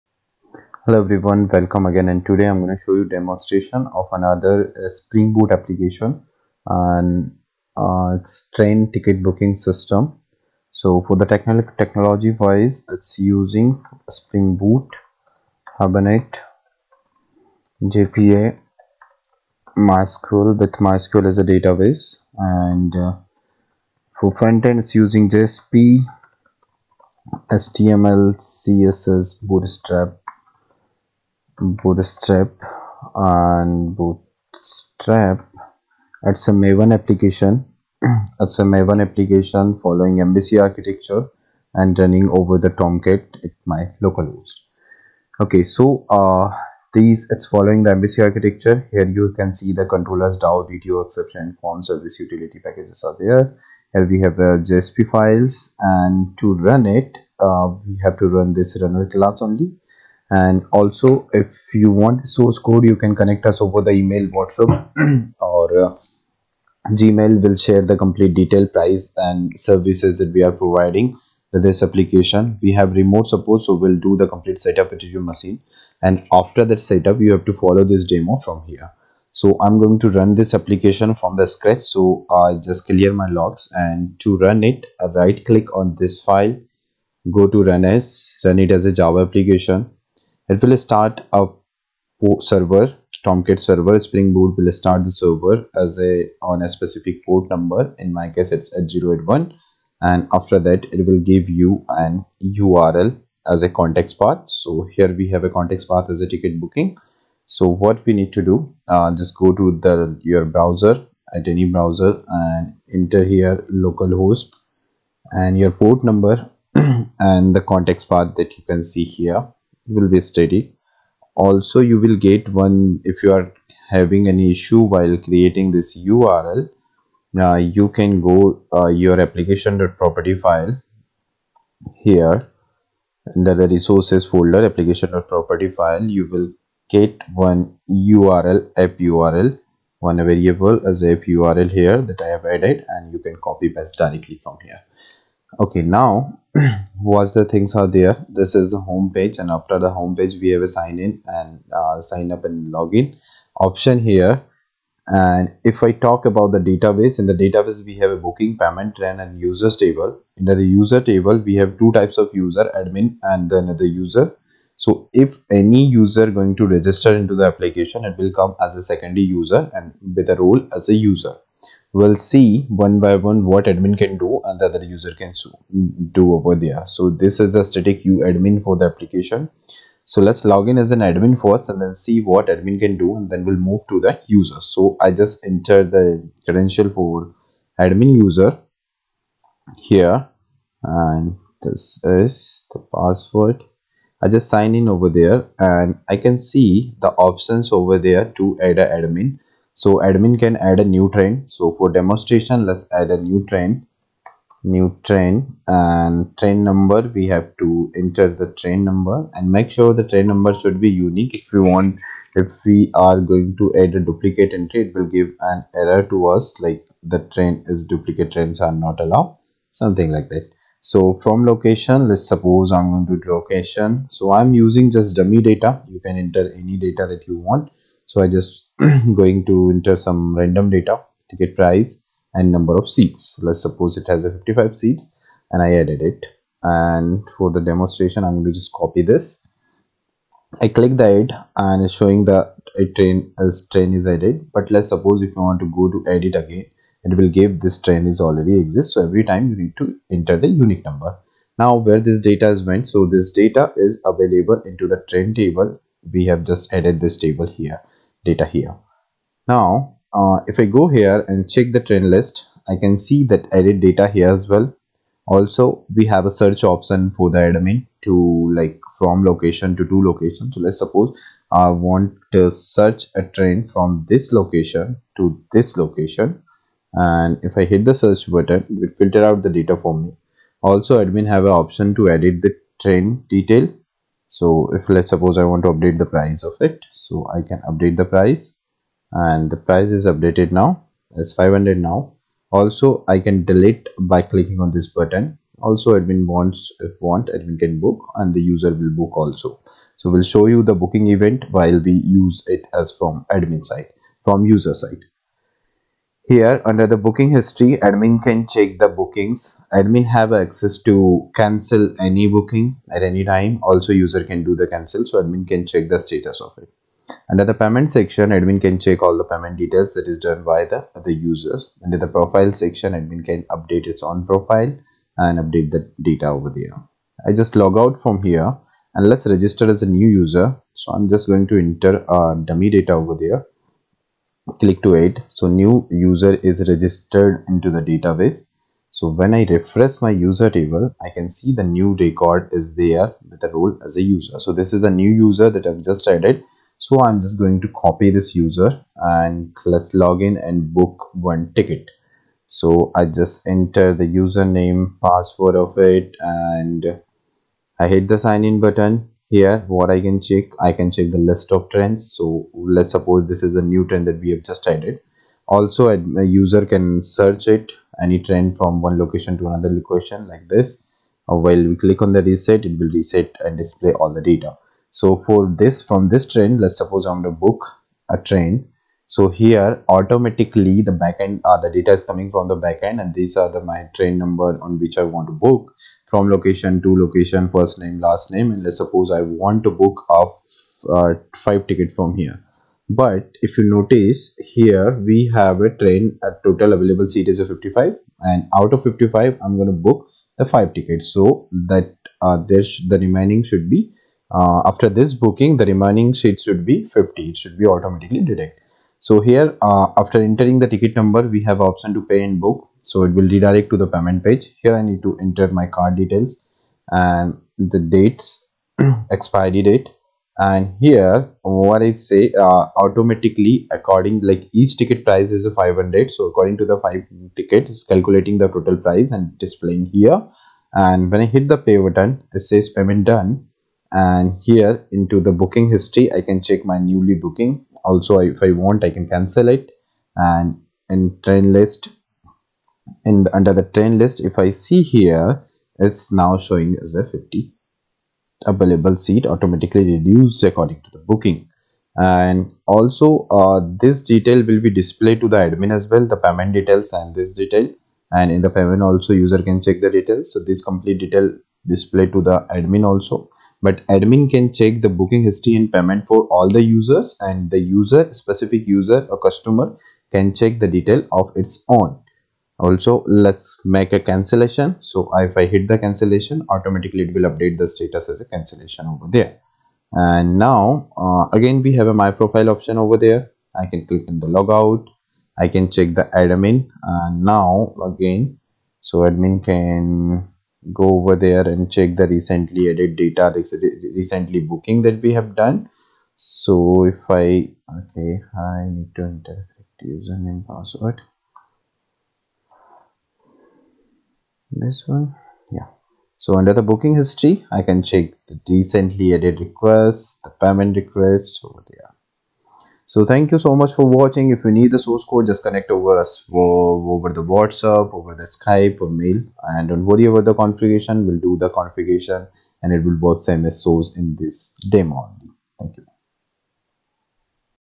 Train Ticket booking project in Spring boot, Hibernate, JPA, and MYSQL with source code
Online train reservation or train booking complete project in Java using spring boot, hibernate, JPA with MySQL